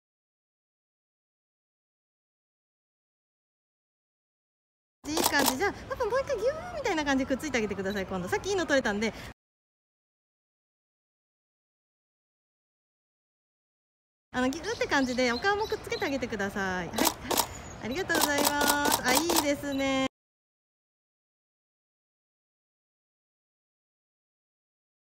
増上寺 お宮参り 出張撮影
増上寺へのお宮参りに同行&出張撮影させていただきました。